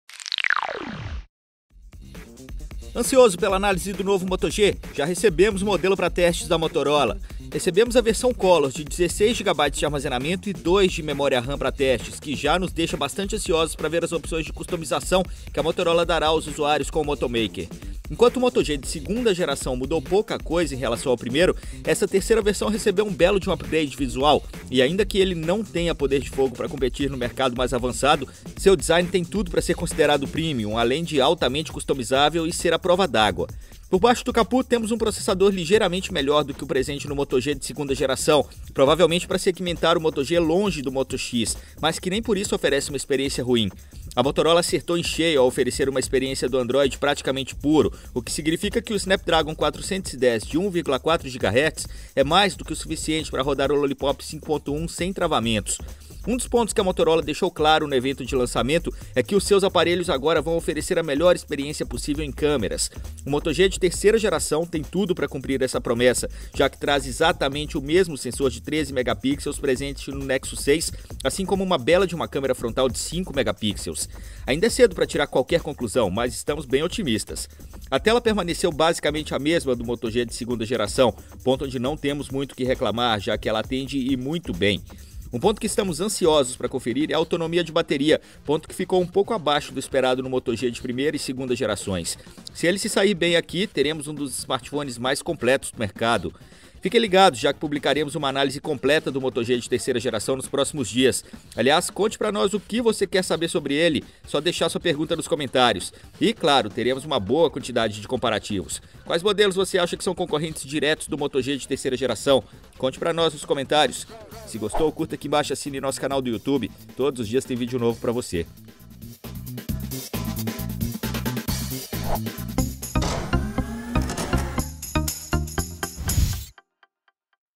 Moto G 2015 (3ª geração) [Hands-on]
Ansioso pela análise do novo Moto G? Já recebemos um modelo para testes da Motorola - a versão Colors de 16 GB de memória interna e 2 GB de RAM, que já nos deixa bastante ansioso para ver as opções de customização que a Motorola dará aos usuários com o Moto Maker.